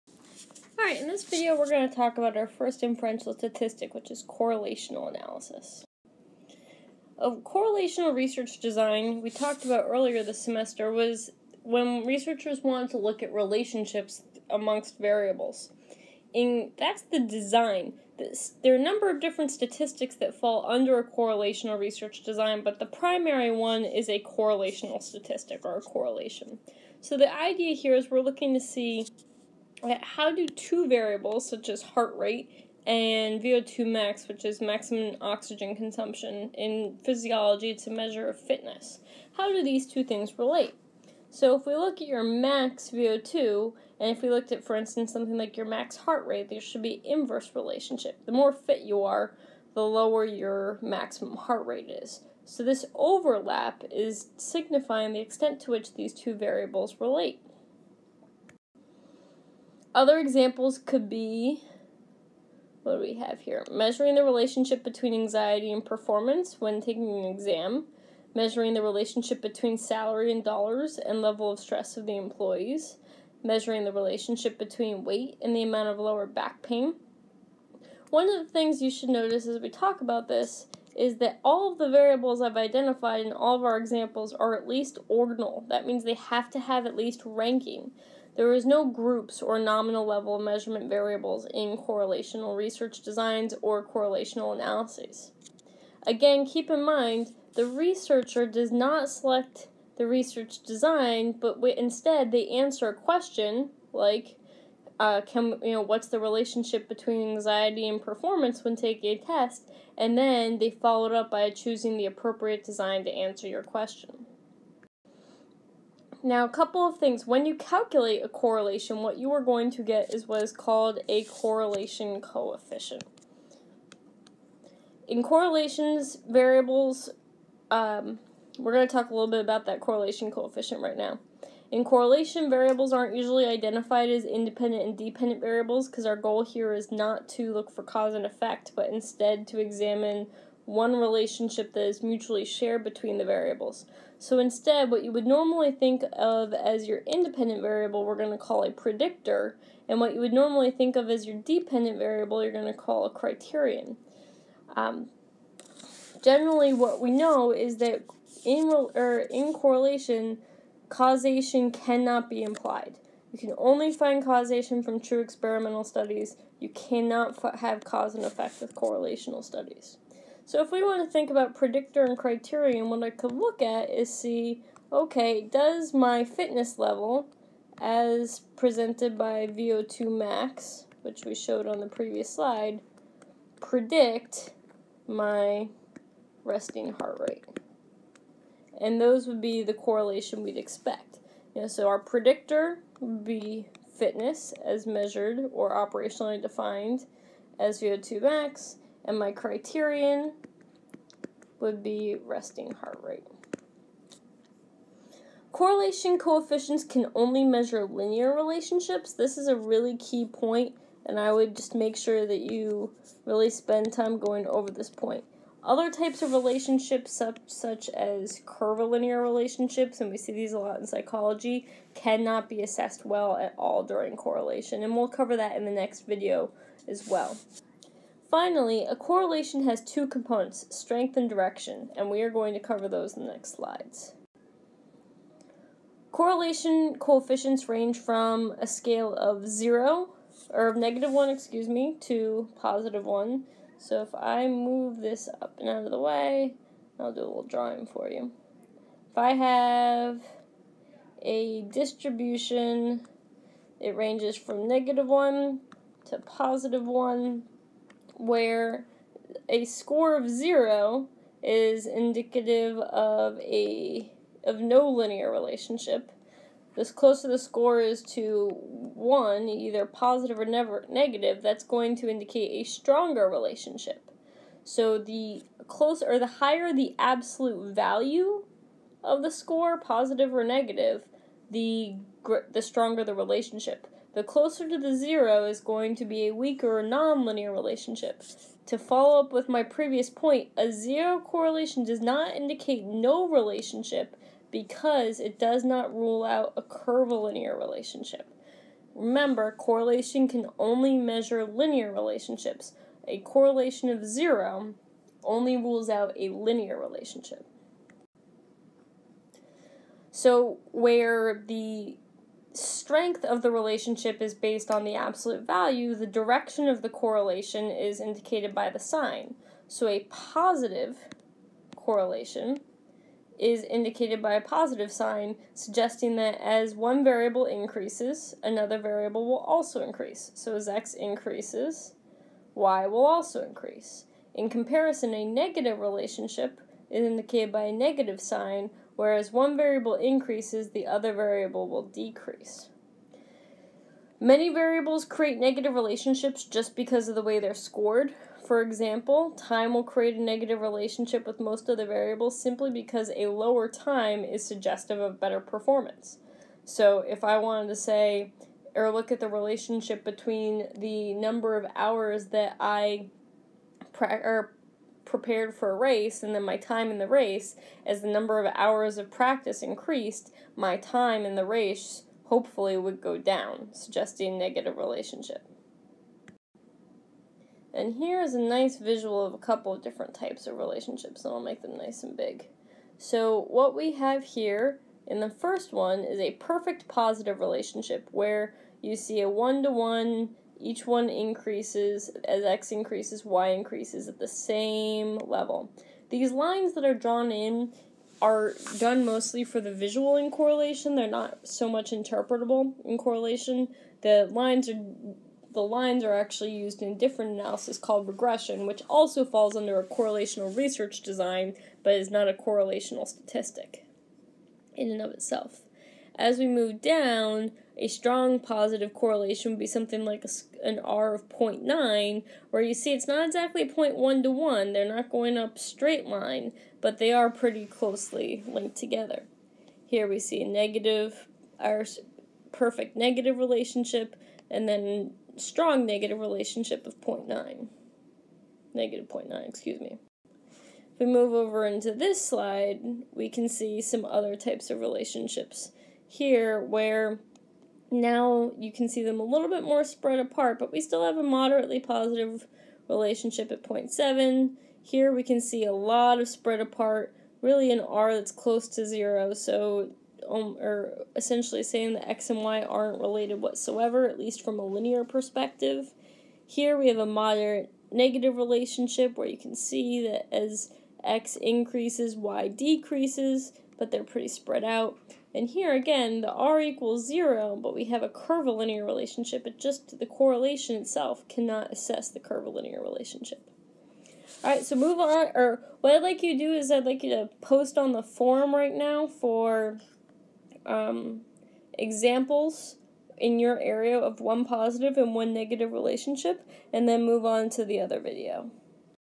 Correlational Analysis
Describes the basics of a correlation coefficient